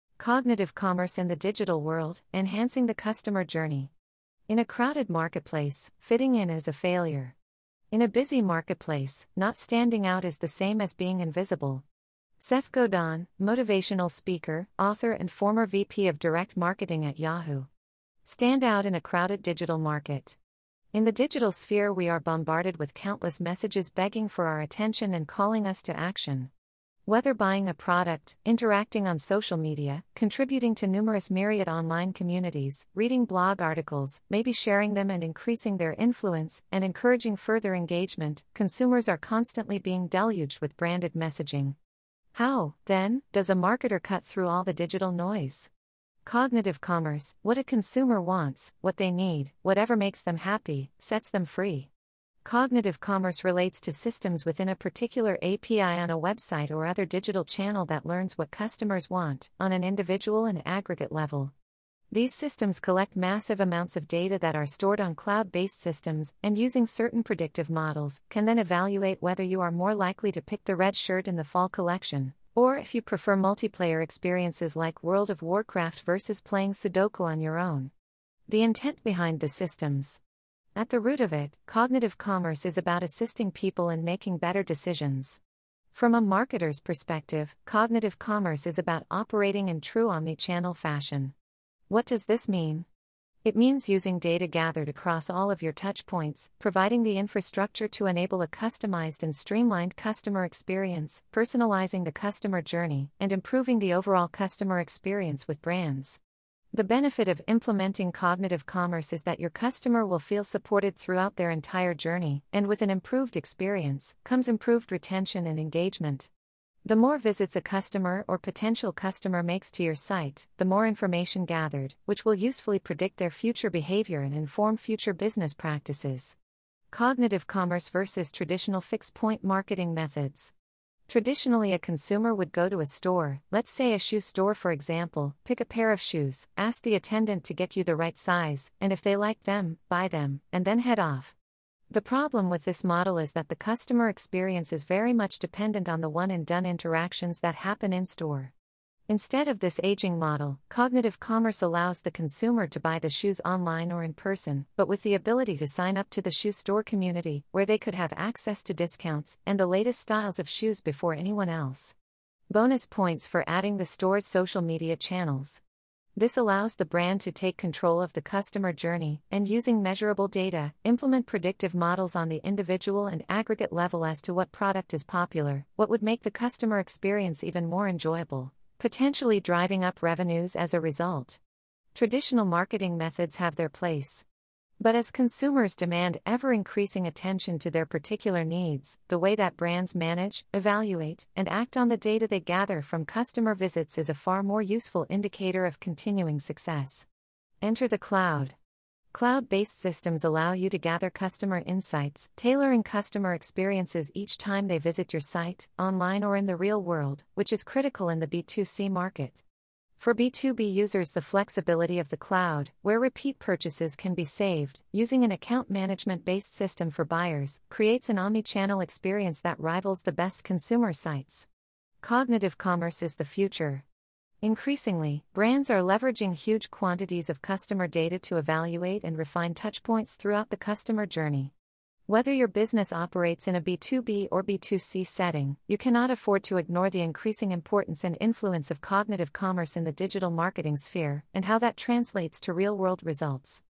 Cognitive commerce in the digital world: Enhancing the customer journey
Engage and support your customers throughout their journey using cognitive commerce and watch your profits grow.